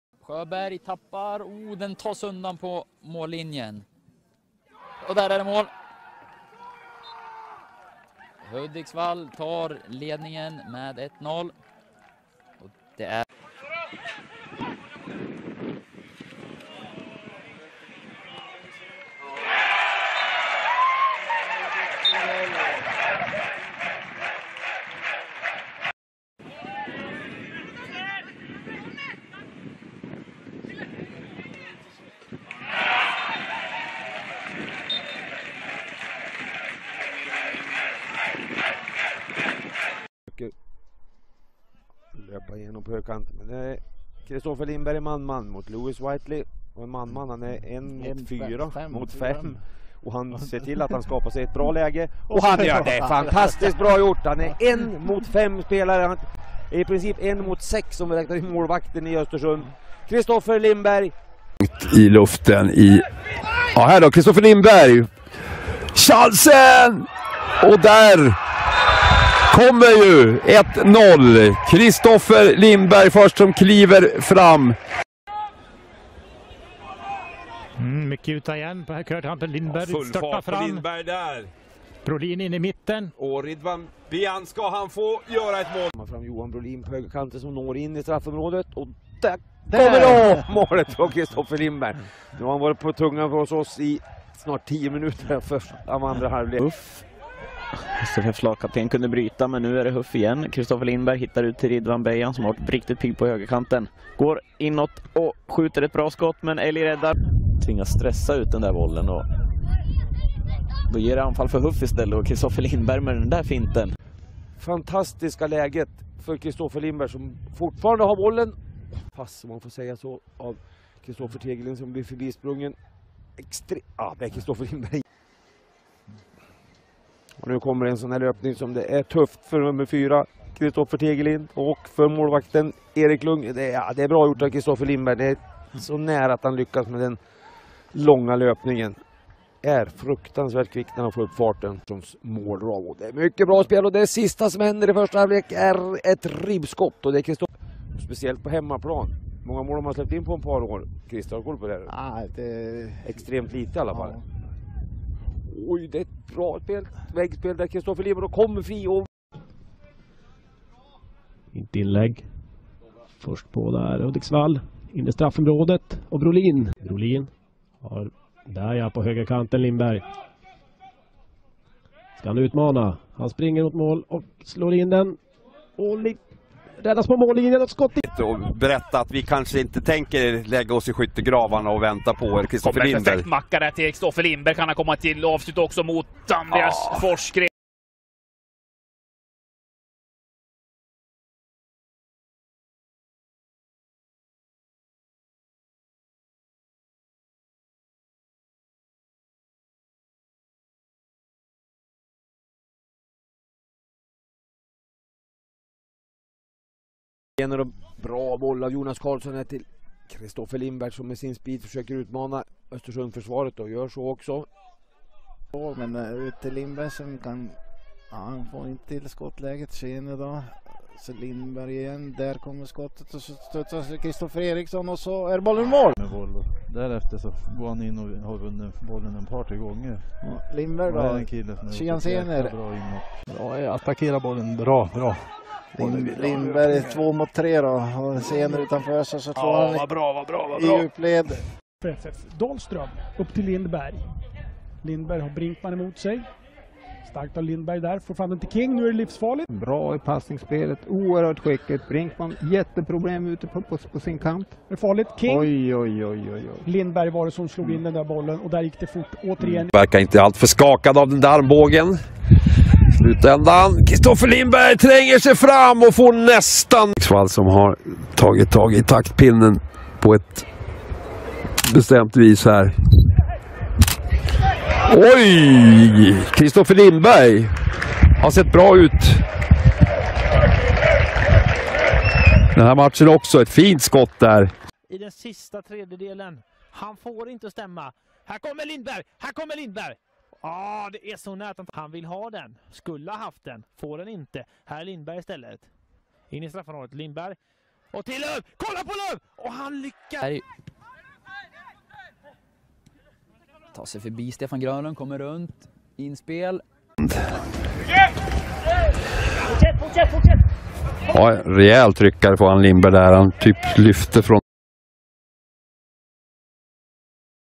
Christoffer Lindberg Hudiksvalls FF
Mål/assist och en del annat från säsongen i Hudiksvalls FF, division 2 norrland. Klipp från Team Stream, Mitt Media och Hela Hälsingland.